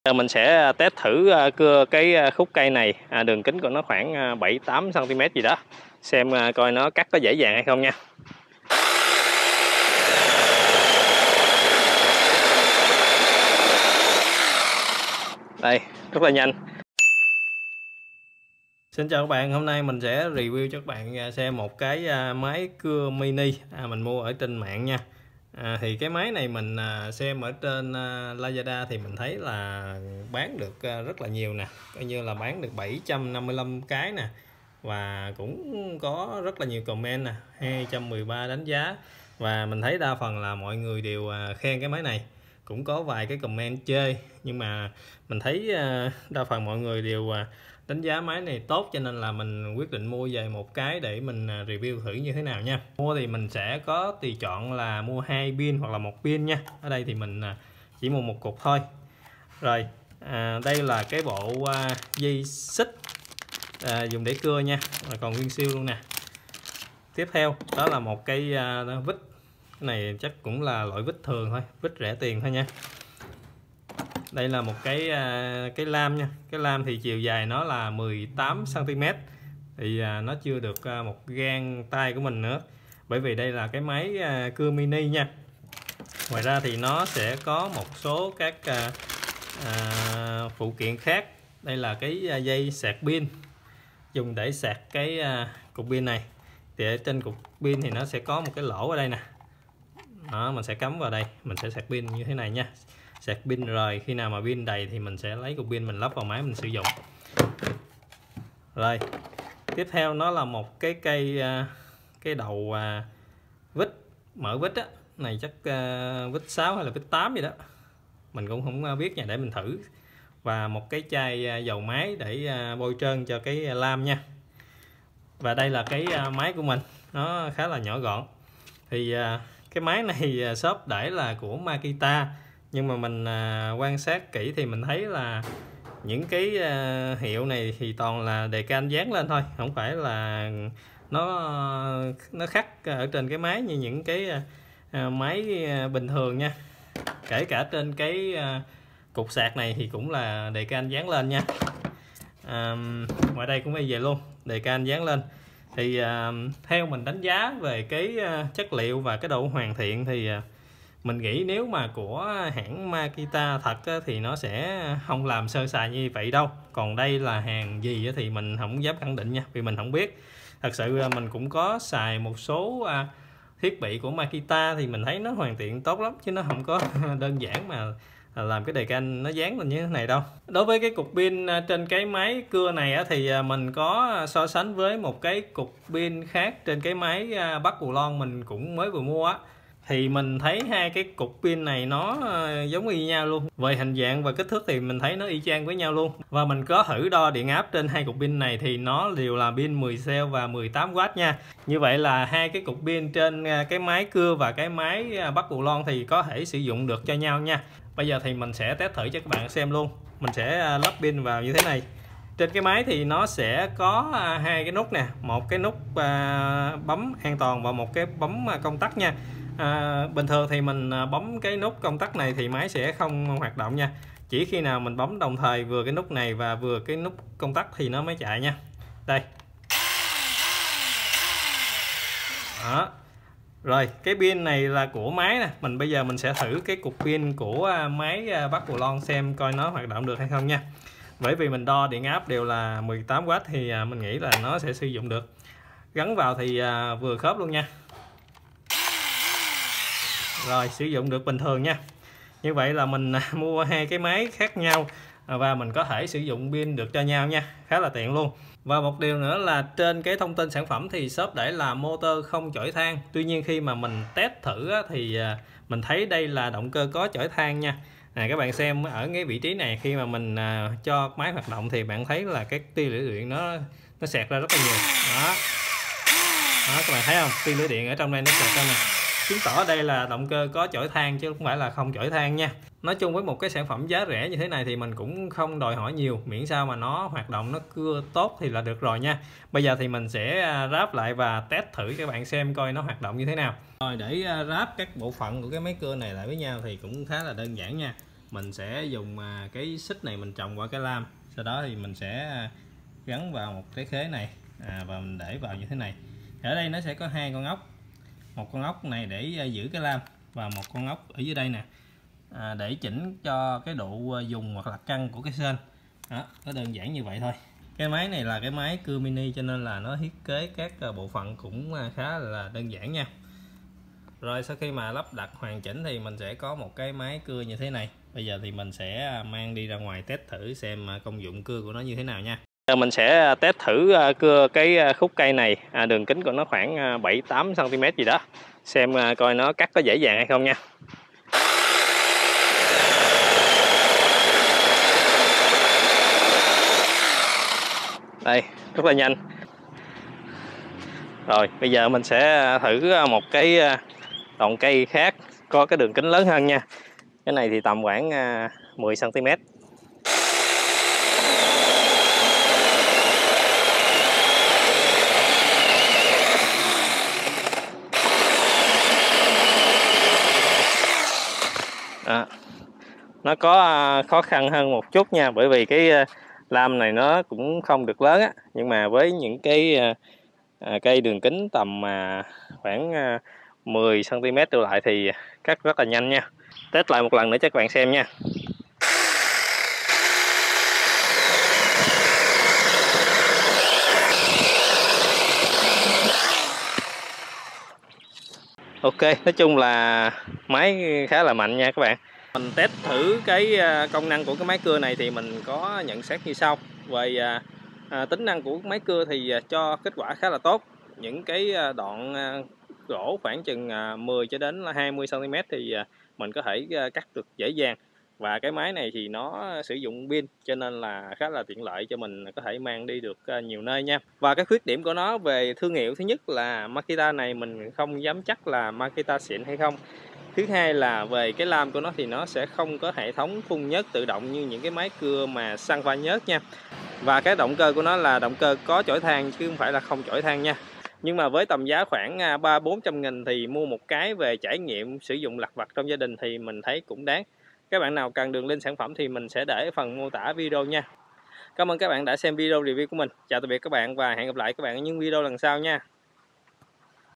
Test thử máy cưa cầm tay Makita mua trên mạng và cái kết
Mình đã test thử máy cưa cầm tay mini Makita mua trên Lazada kết quả như sau:
Ưu điểm:
- Máy cưa cầm tay mini MakitaMáy nhỏ gọn, sử dụng pin sạc, tiện dụng mang đi theo người.
- Chức năng cưa rất ngon, phù hợp cưa cây đường kính nhỏ hơn 20 cm
Khuyết điểm:
- Máy cưa cầm tay mini Makita này là hàng gi chưa kiểm chứng sịn hay không
- Động cơ không có phun nhớt bôi trơn lam
- Động cơ có chổi than
Cám ơn các bạn đã ủng hộ kênh Thích Làm Điện